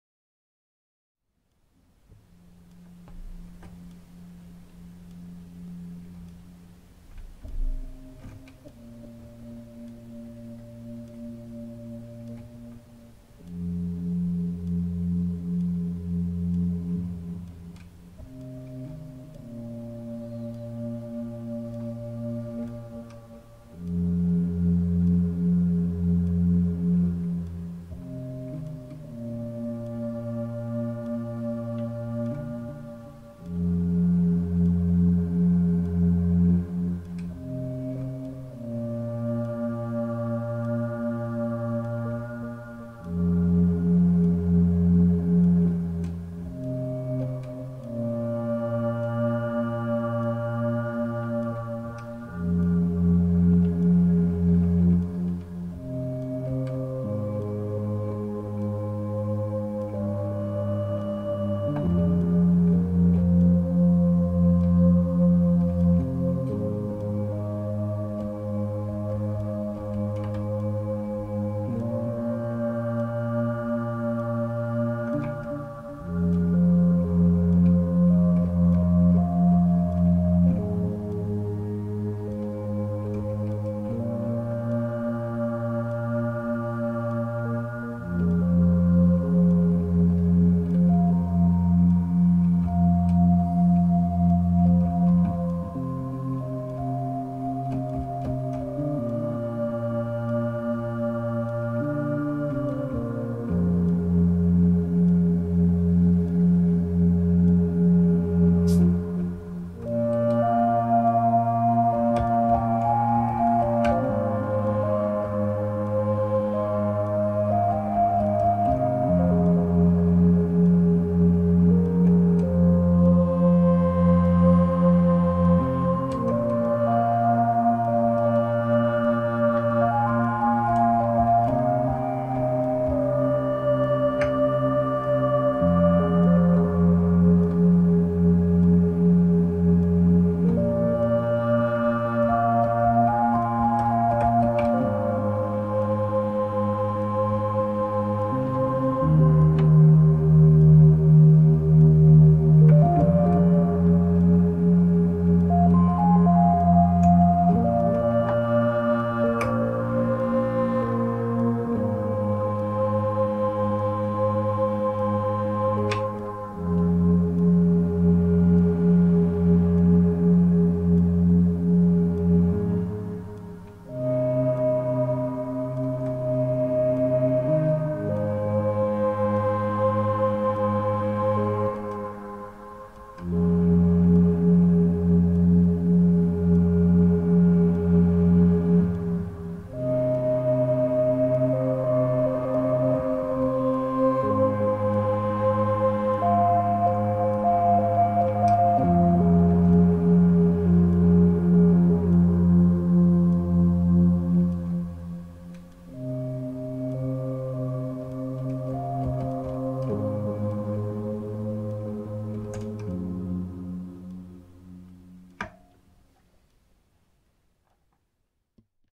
Ambient Sketch (Melancholic) feat. Behringer Neutron & Analog FX Pedals
A short sketch. Working the Neutron for my 3rd mini-album. Exploring it in combination with some characterful analog guitar pedals. I liked the atmosphere.

There seems to be some sort of clipping going on. Neverthless I chose to keep it. Could also be a result of the overdrive of the +1 osc on the KMA Moai Maea pedal.

Recorded with a Zoom stereorecorder, from the monitors. Some noise reduction done but since i am from the cassette-tape-era I sometimes find noise a pleasant background-presence.